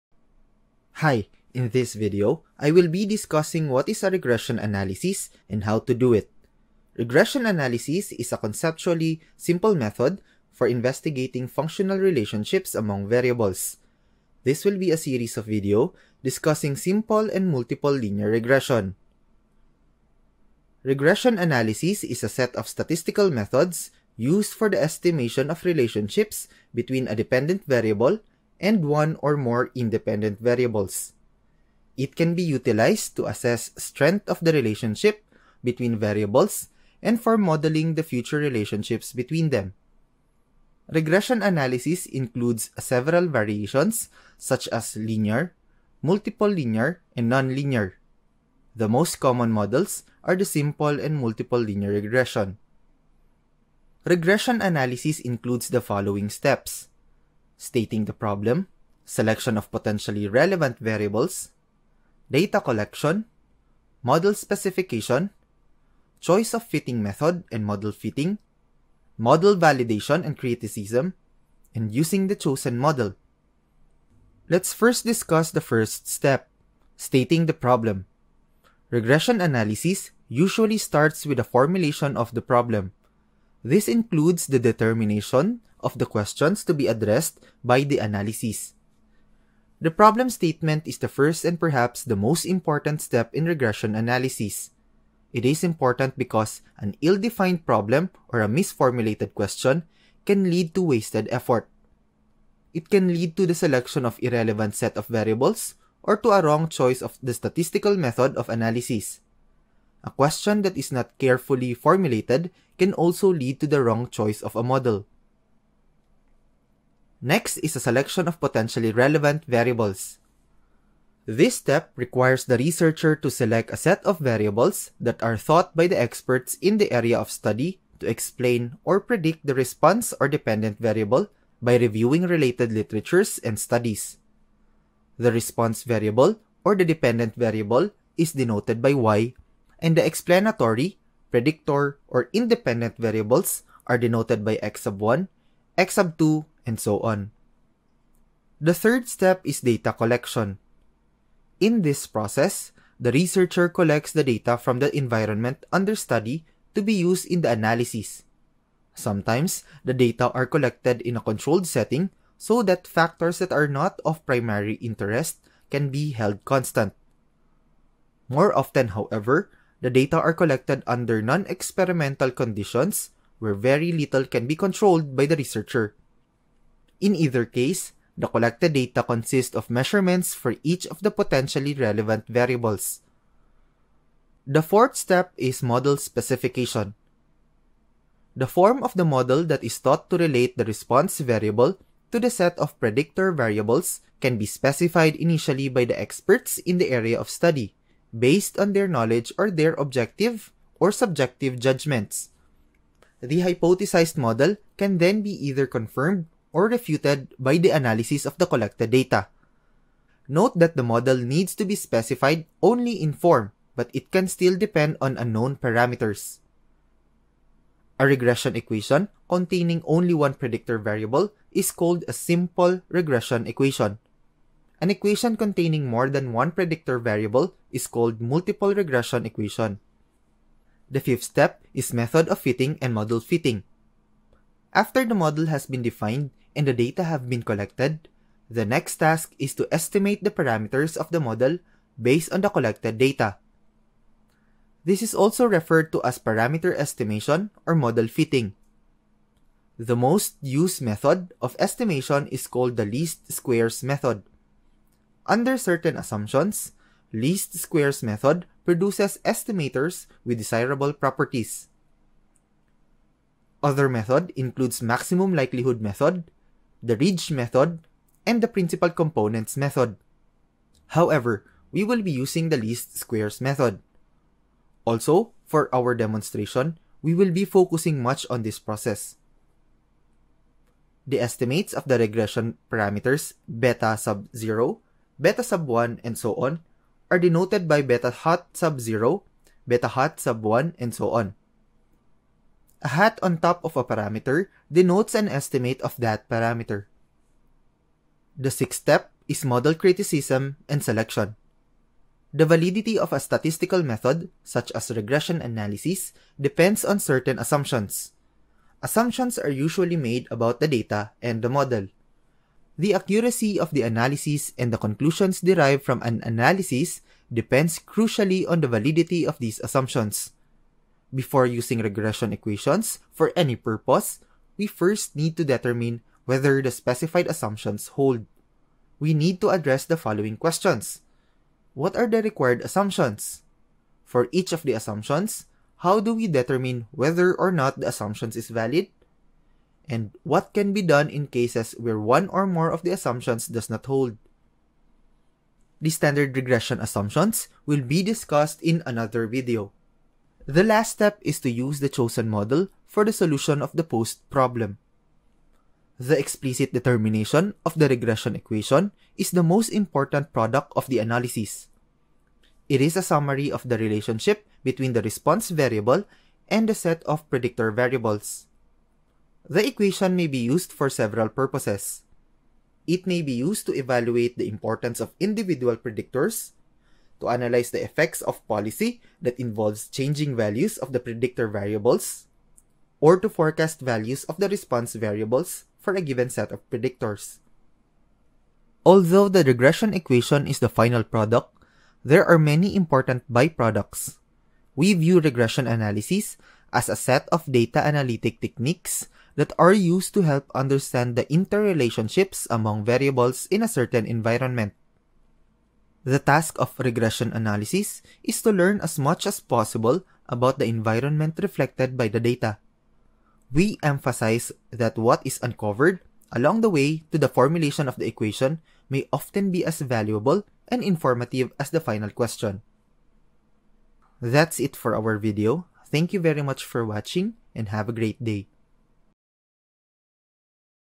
Quantitative Research | Regression Analysis
In this video, I will be discussing what is a regression analysis and how to do it. Regression analysis is a conceptually simple method for investigating functional relationships among variables. This will be a series of video discussing simple and multiple linear regression.